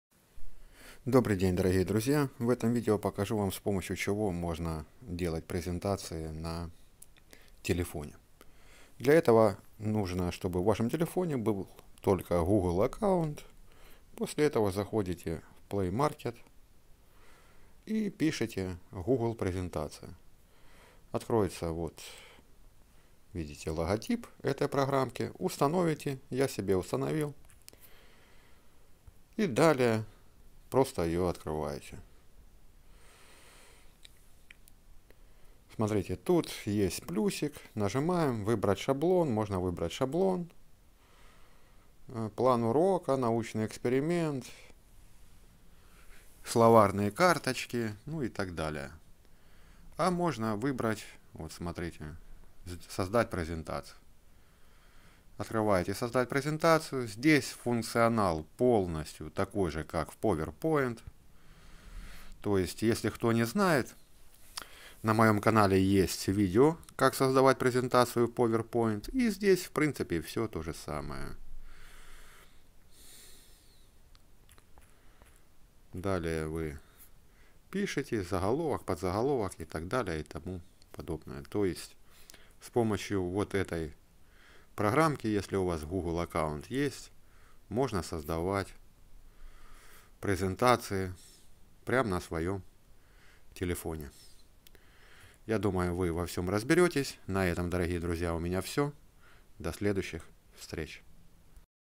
Как сделать презентацию на телефоне
В приложении Google Презентации для Android можно создавать, просматривать и редактировать презентации Google и Microsoft PowerPoint®.